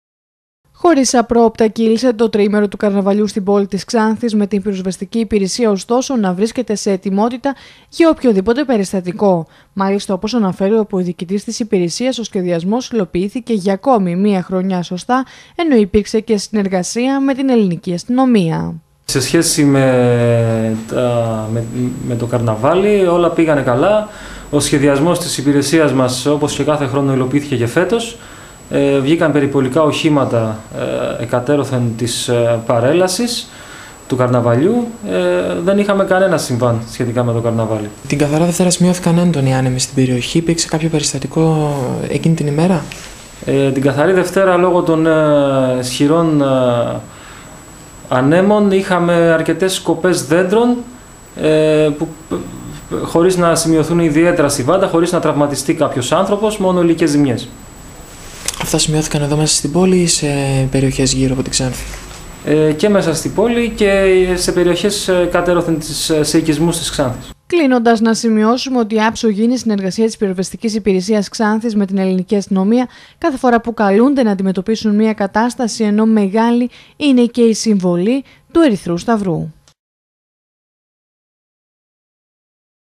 ΧΩΡΙΣ ΣΟΒΑΡΑ ΠΕΡΙΣΤΑΤΙΚΑ ΤΟ ΤΡΙΗΜΕΡΟ ΤΟΥ ΚΑΡΝΑΒΑΛΙΟΥ ΣΤΗΝ ΞΑΝΘΗ
ΧΩΡΙΣ ΣΟΒΑΡΑ ΠΕΡΙΣΤΑΤΙΚΑ ΤΟ ΤΡΙΗΜΕΡΟ ΤΟΥ ΚΑΡΝΑΒΑΛΙΟΥ ΣΤΗΝ ΞΑΝΘΗ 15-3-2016